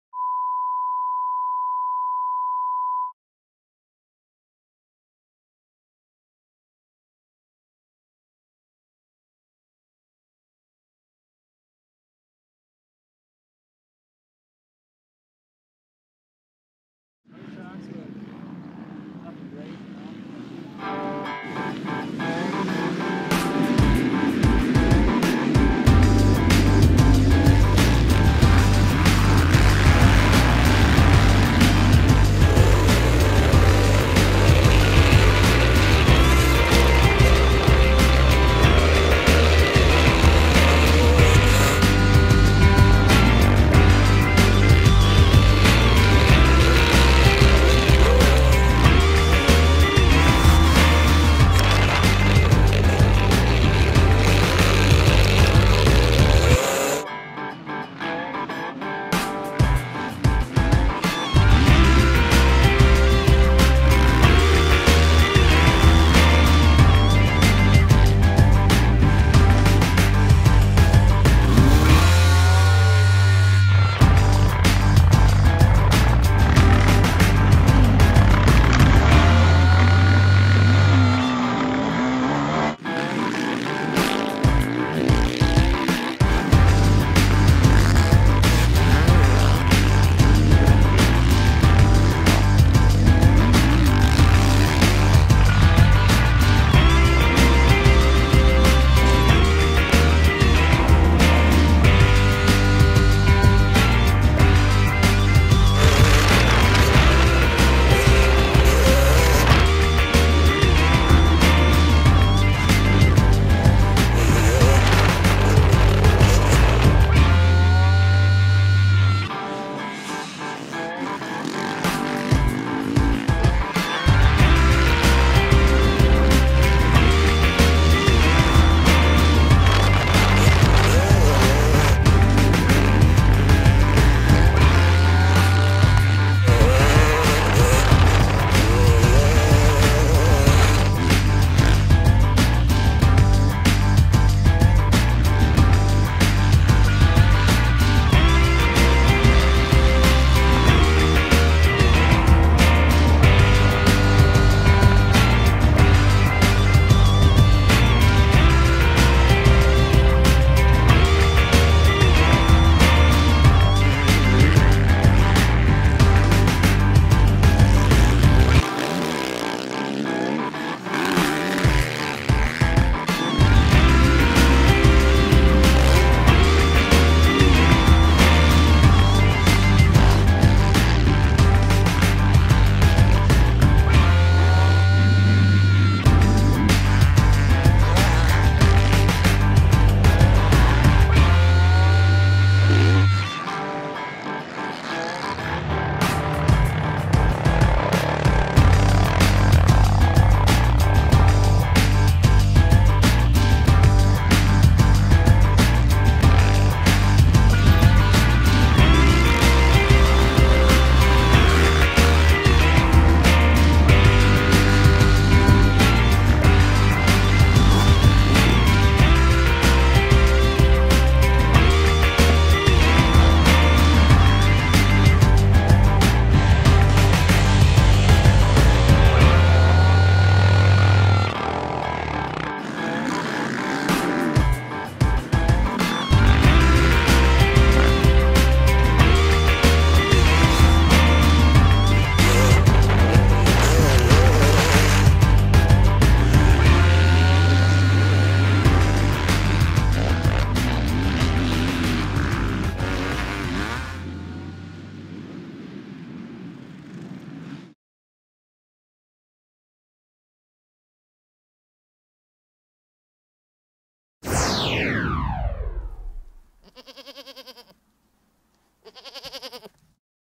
Hemonds Montage.mov
This was a piece that I put together for a local cable show.  Rider is #909 Ryan Wadsworth from Enfield CT. The track is Hemond's MX Park in Minot, ME